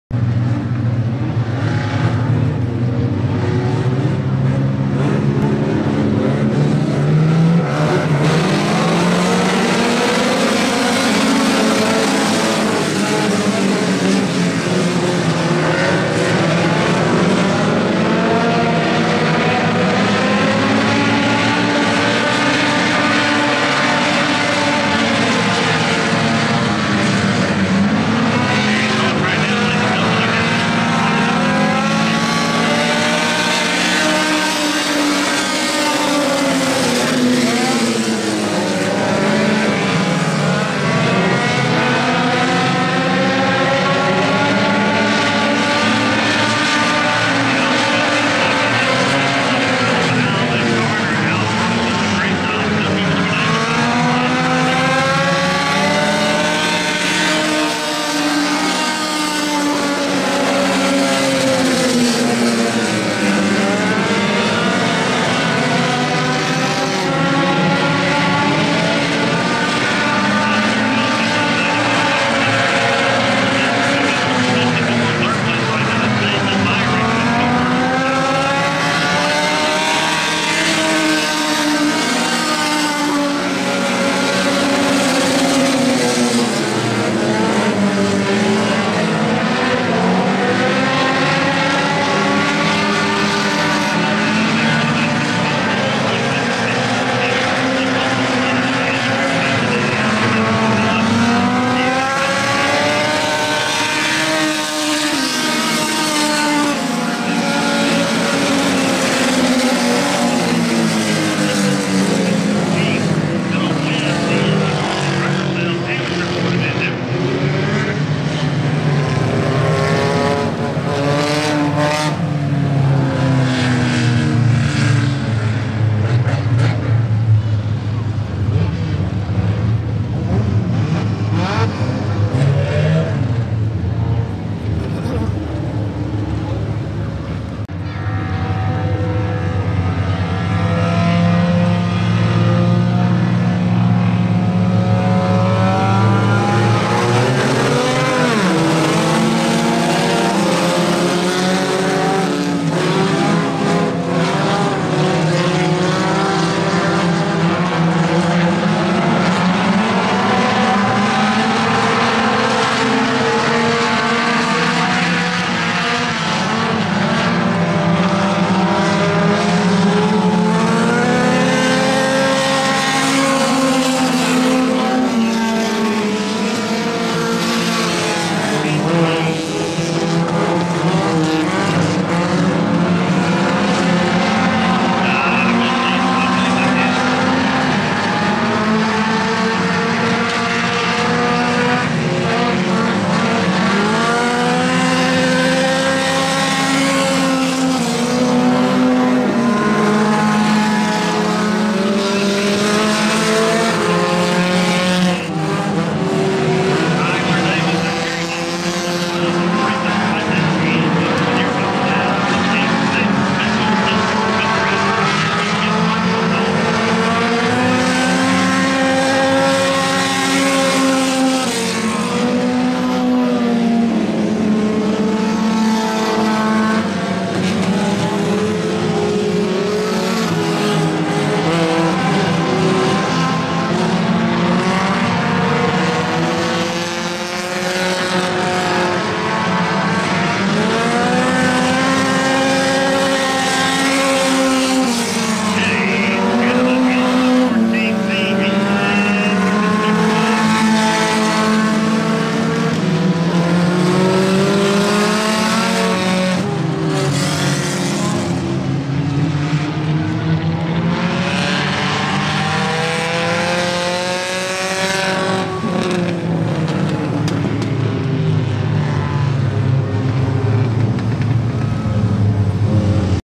02/26/21 Super 4 Heat Races - Oglethorpe Speedway Park
HEAT RACE 1 - 74 David Myrick, 99 Joseph Thompson, 777 Alan Carter, 007 Justin Stephens, 83 Robbie Strickland, 69 Garrett Norton, 04 Tyler Davis, 614 Tony Wilson, 77 Josh Humrichhouse
HEAT RACE 2 - 14c James Canada, 19 Ryan Norton, 189 Rafael Dejesus, 999 Jeremiah James, 5 Terry Davis, 86 Mitchell McNure, 02 Bradley Bratcher, 68 Rick McNure